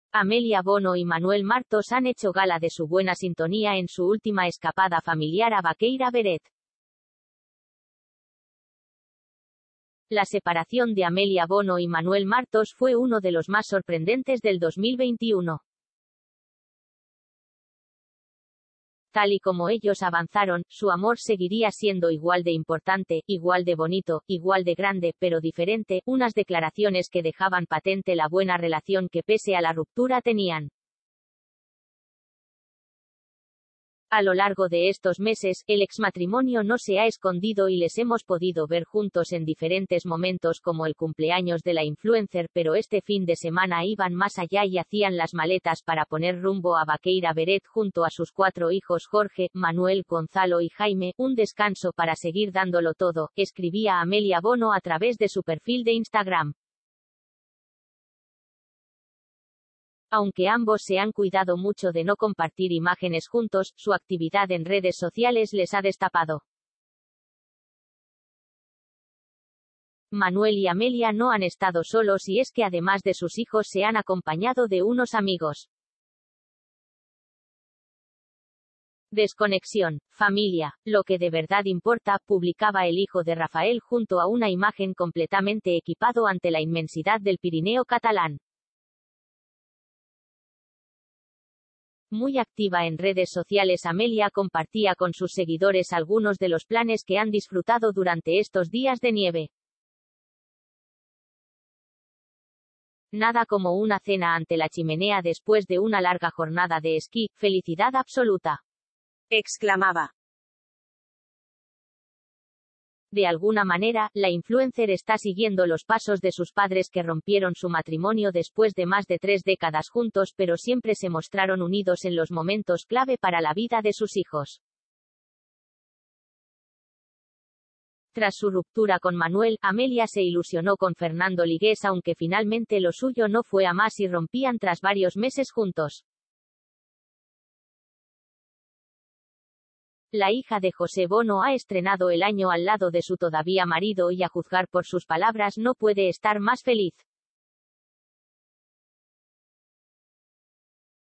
💛Amelia Bono y Manuel Martos, juntos en Baqueira Beret con sus cuatro hijos
Amelia Bono y Manuel Martos han hecho gala de su buena sintonía en su última escapada familiar a Baqueira Beret